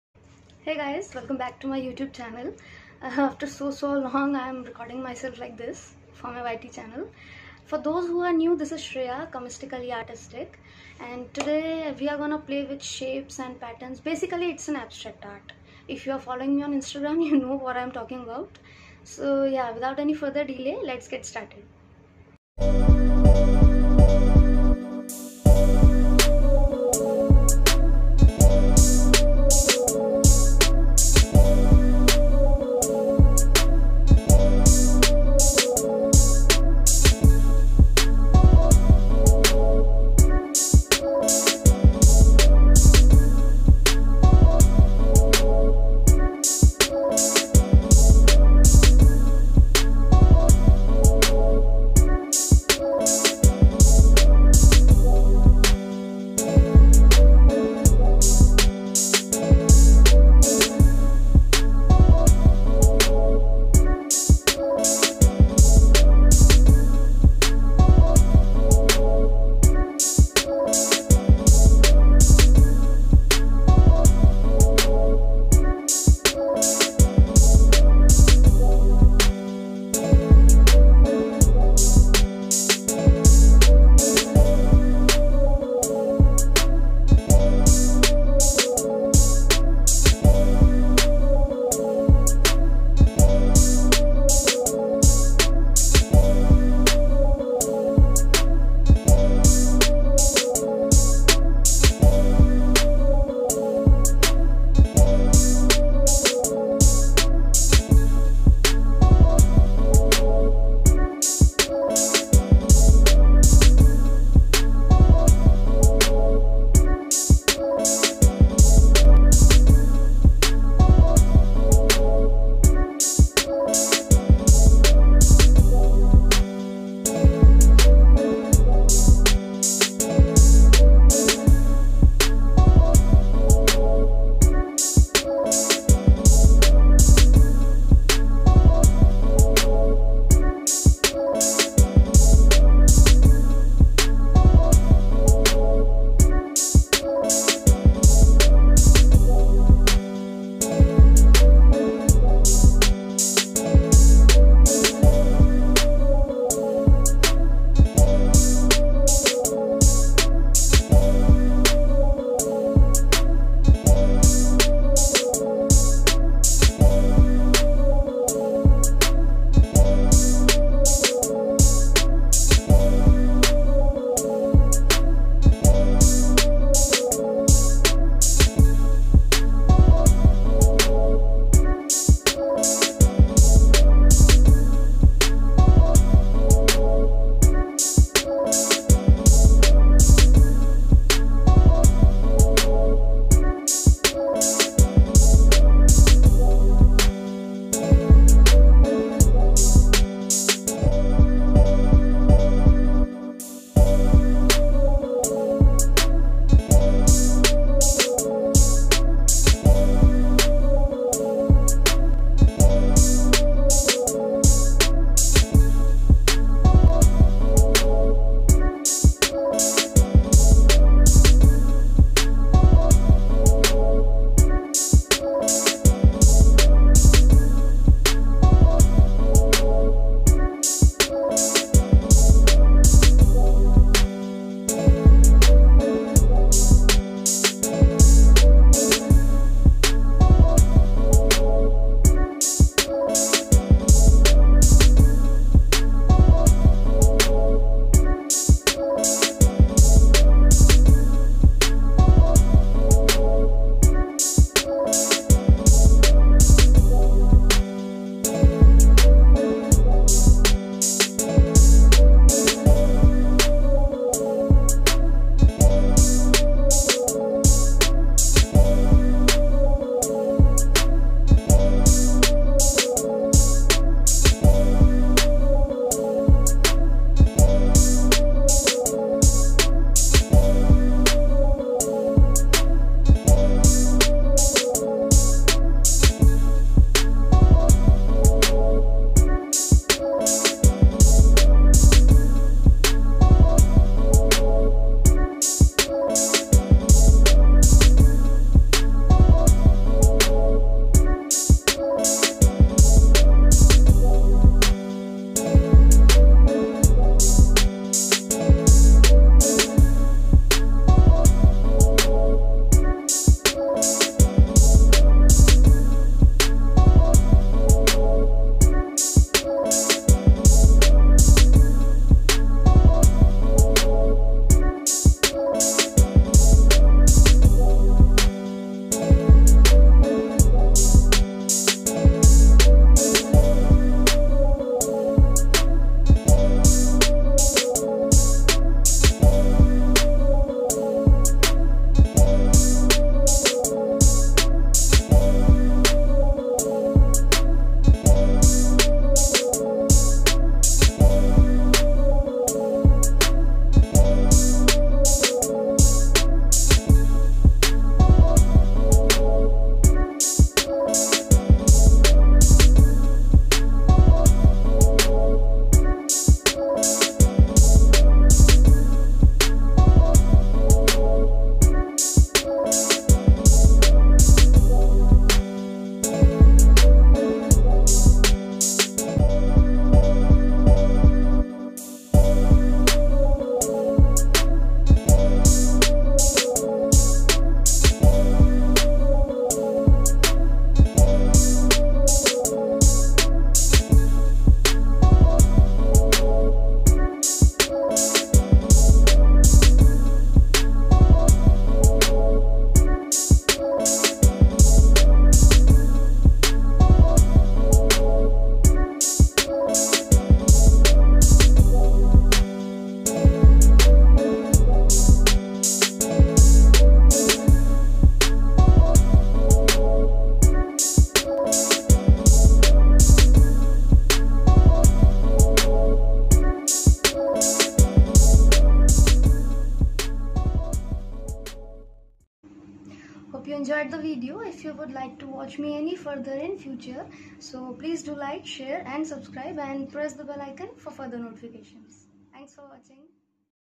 Abstract art for beginners/ Abstract Painting/ Easy Abstract Painting/
Here's a quick tutorial video of easy Abstract Painting for beginners!🎨
Materials Used:
Camlin Premium Poster Colours
Doms Watercolour Cakes
Sakura gelly roll moonlight gel pen(white08)
Luxor Fine Writer (Black)
Menorah 180gsm square sketchbook
Wanna catch the glimpses of my works then do check out my